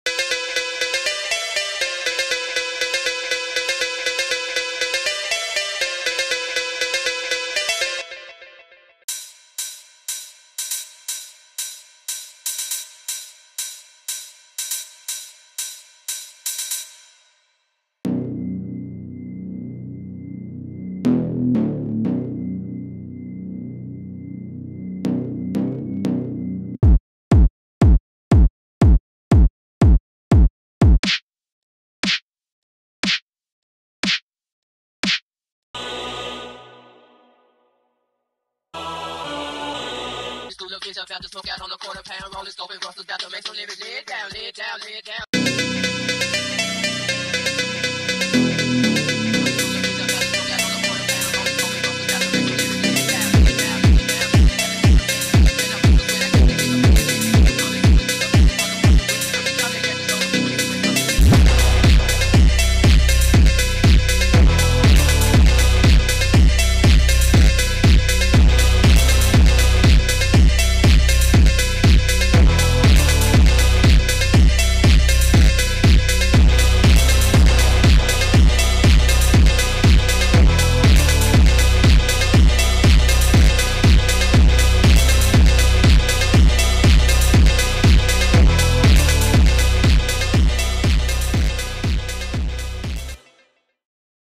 [FREE KIT] HOW TO MAKE PHONK LIKE KORDHELL, DXRK & KUTE
HOW TO MAKE PHONK LIKE KORDHELL DXRK & KUTE

In this small video, I'll show how to make phonk like kordhell, dxrk & kute. It's actually pretty easy when you get the hang of it.